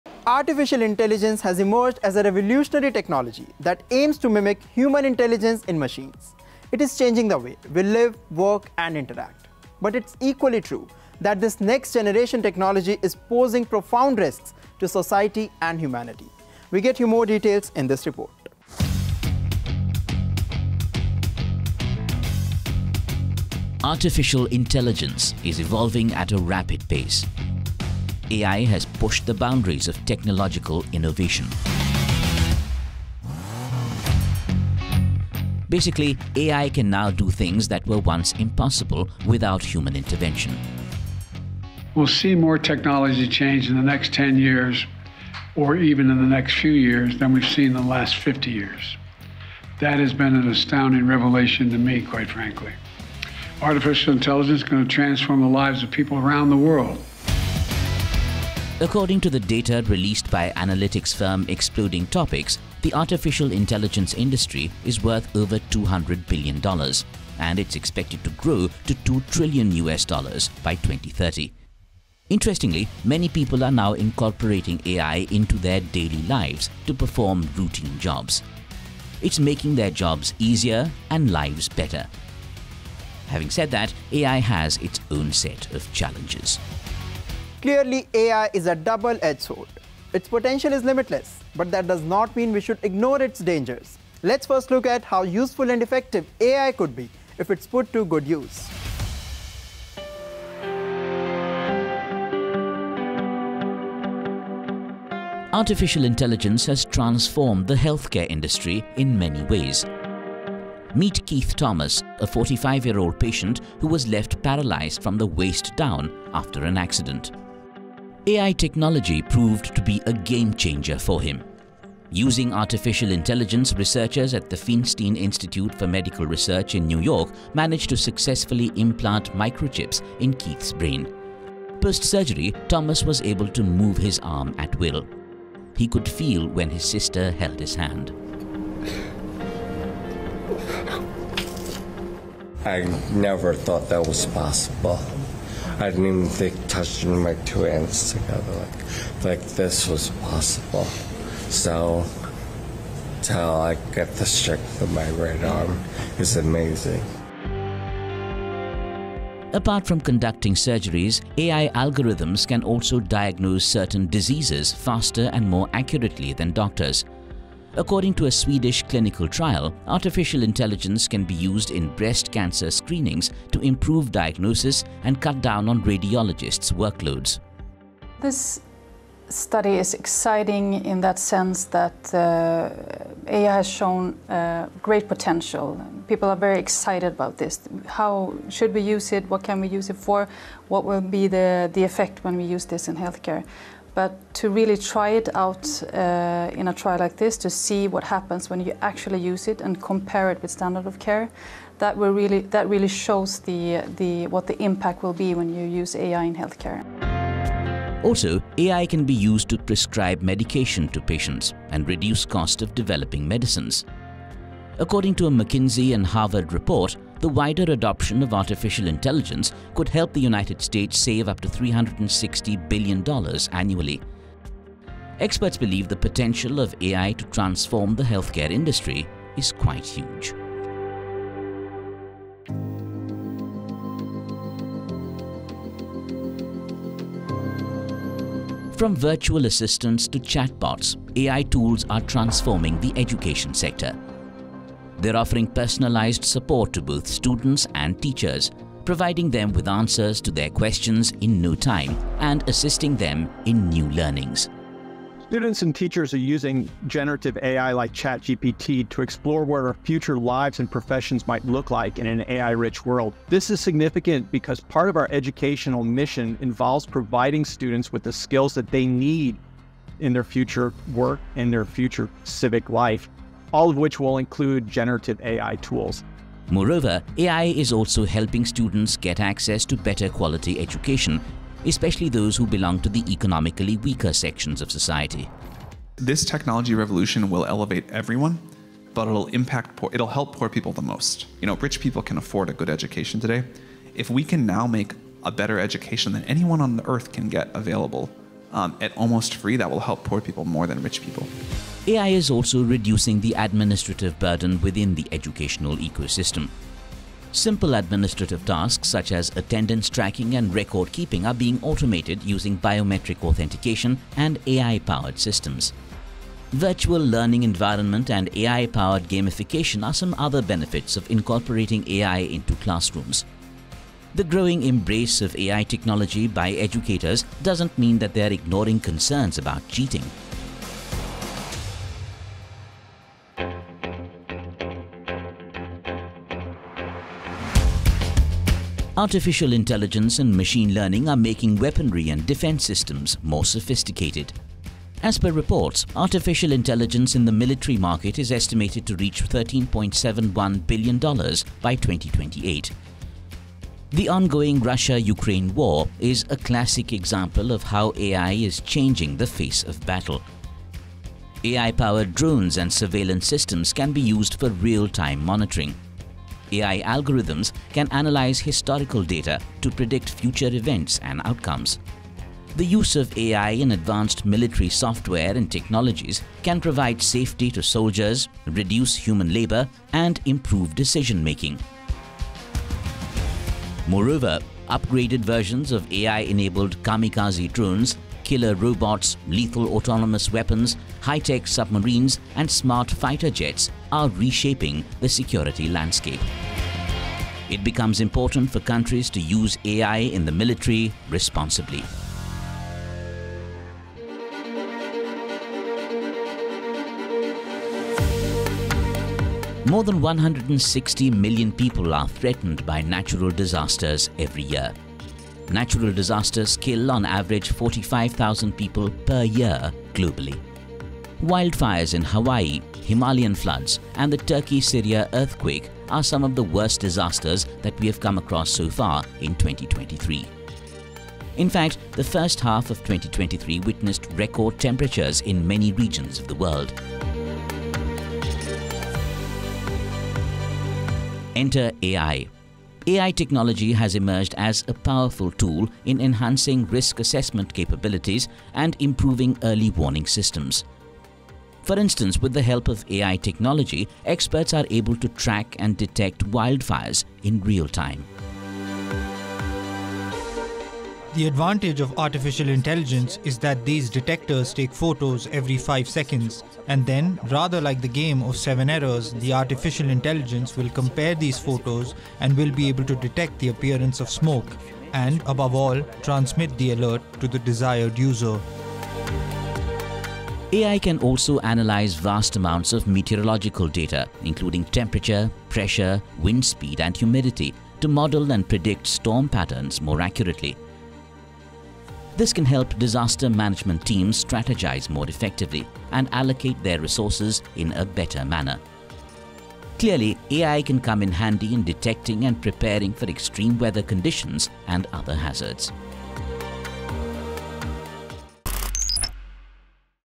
Killer robots to suicide drones: Future of warfare | Tech It Out
Artificial intelligence has emerged as a revolutionary technology that aims to mimic human intelligence in machines. We take a look at how useful and effective AI could be if it's put to good use. Here's more.